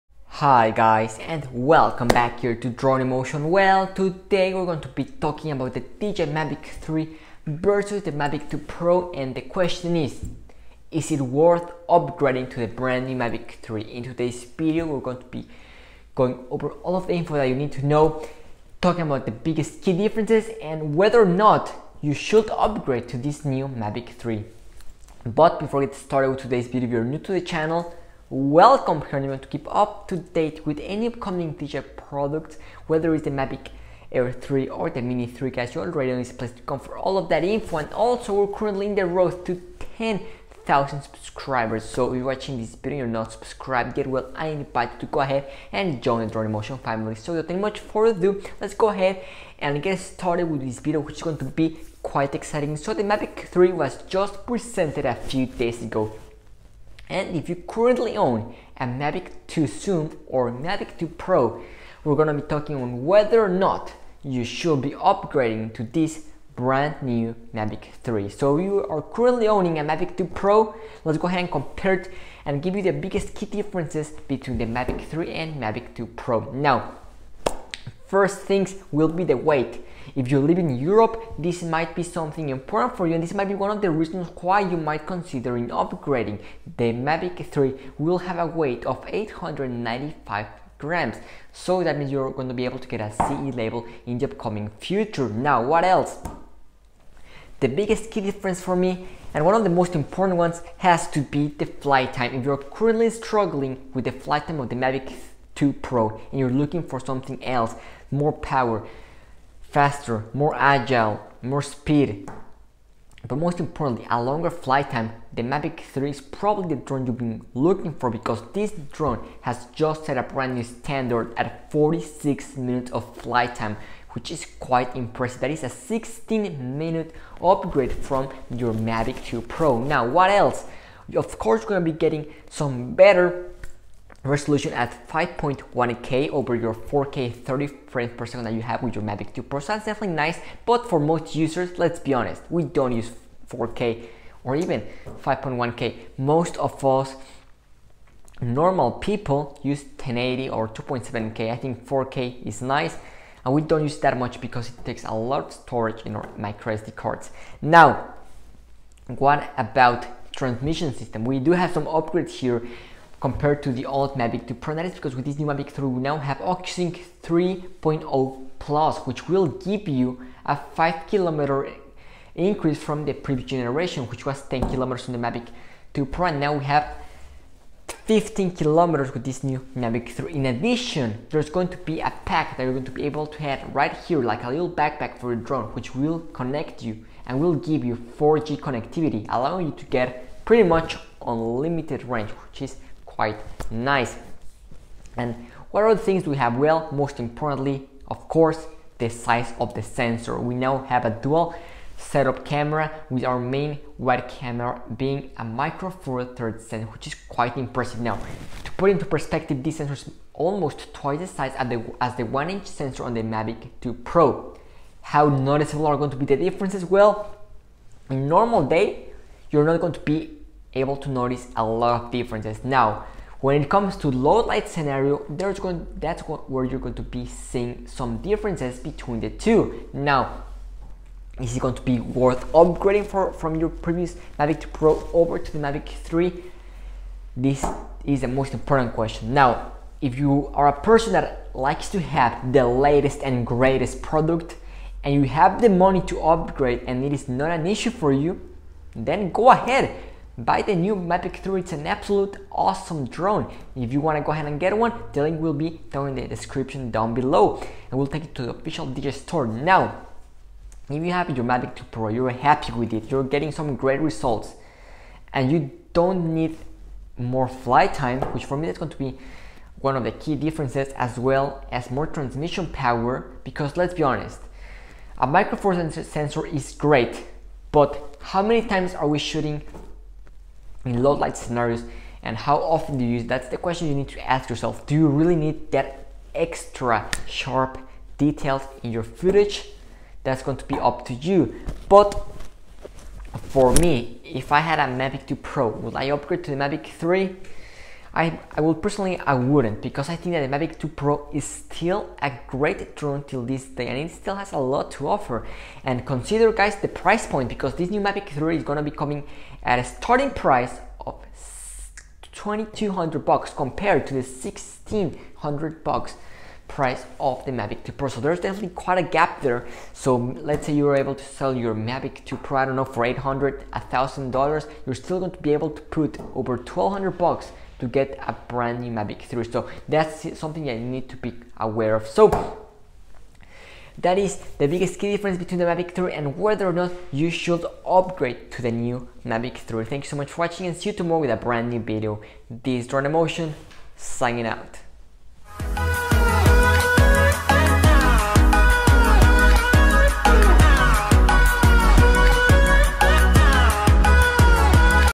DJI Mavic 3 vs Mavic 2 Pro//Should You Upgrade?
In today's video we talk about whether or not you should upgrade from the Mavic 2 Pro to the Mavic 3.